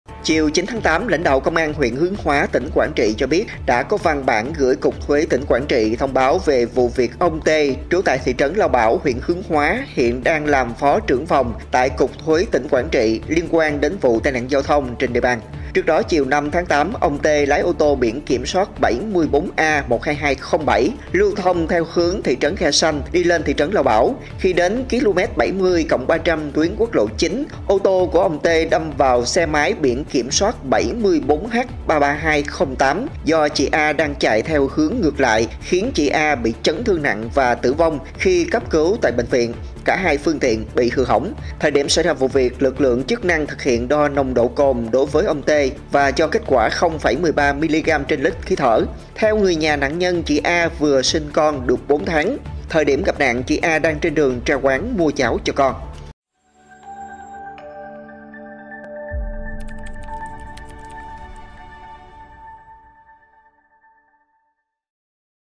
Phó trưởng phòng Cục Thuế Quảng Trị có hơi men lái ô tô gây tai nạn chết người
Chiều 9-8, lãnh đạo Công an huyện Hướng Hóa (tỉnh Quảng Trị) cho biết, đã có văn bản gửi Cục thuế tỉnh Quảng Trị thông báo về vụ việc ông T. (trú tại trú thị trấn Lao Bảo, huyện Hướng Hóa, hiện đang làm Phó Trưởng phòng tại Cục thuế tỉnh Quảng Trị) liên quan đến vụ tai nạn giao thông trên địa bàn.